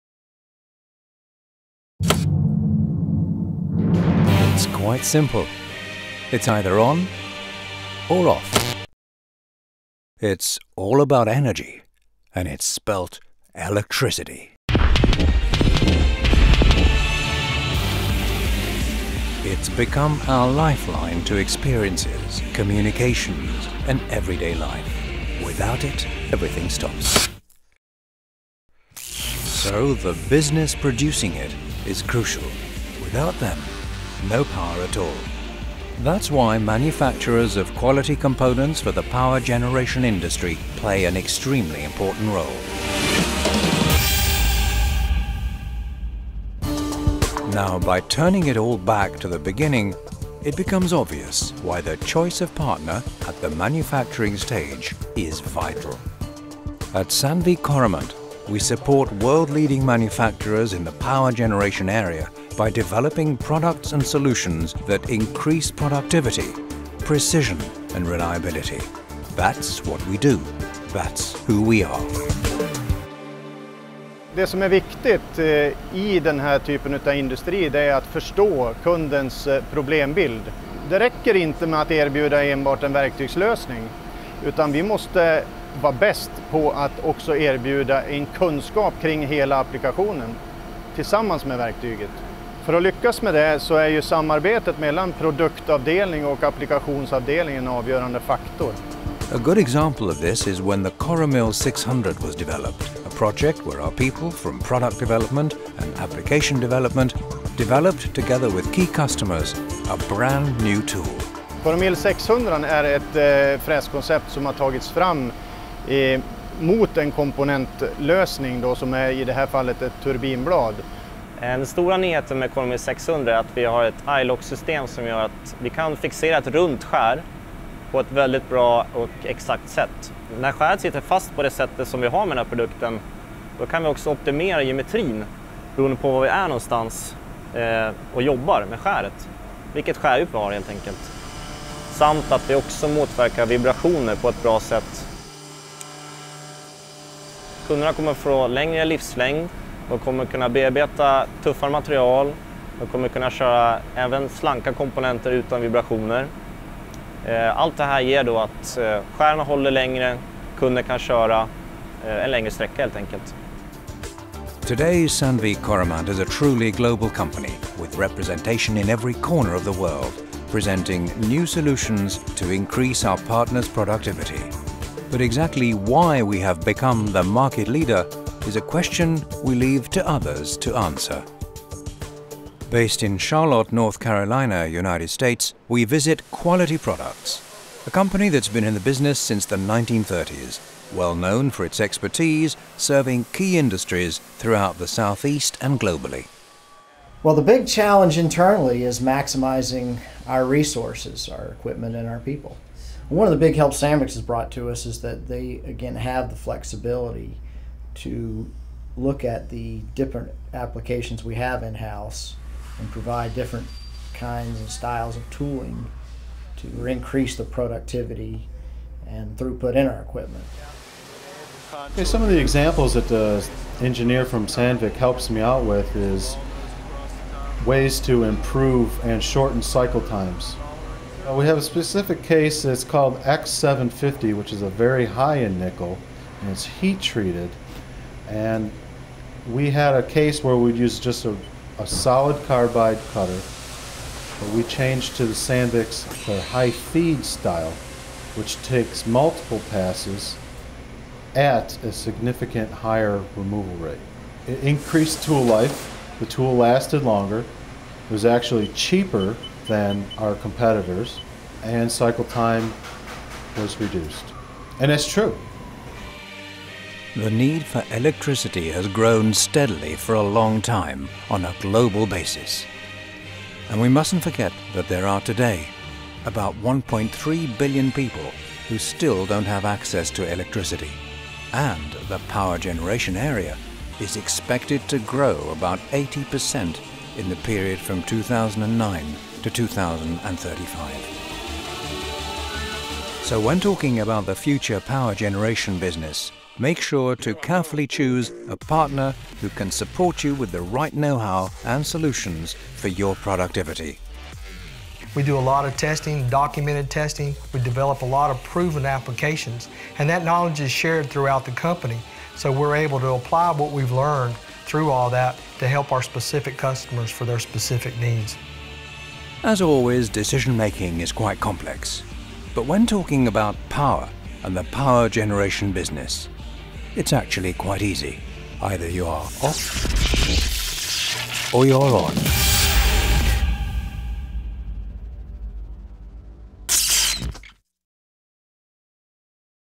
Power Generation -- Sandvik Coromant & USA customer Quality Products
At Sandvik Coromant we support world leading manufacturers in the Power Generation area by developing metal cutting products and solutions that increase productivity, precision and reliability. Quality Products from Charlotte USA have been in the energy business since the 1930s. Here they talk about how they are always looking for new ways to reduce their cycle time.